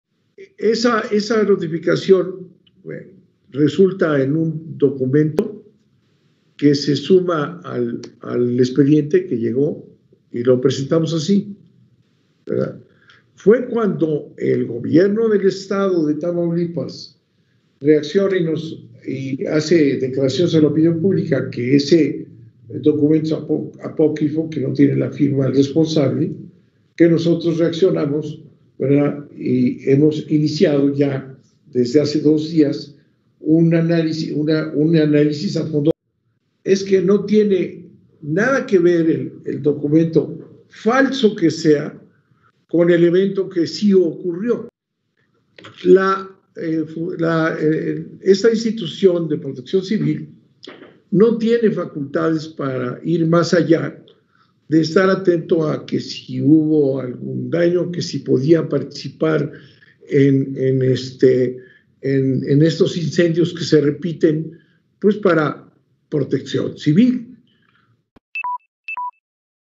Protección Civil de Tamaulipas NO tiene facultades para demandar: Bartlett 🦖
Manuel Bartlett así como cuando salió a decir que se cayó el sistema, hoy declara que no importa tanto el documento apócrifo que la CFE presentó y que Protección Civil de Tamaulipas NO tiene facultades para demandar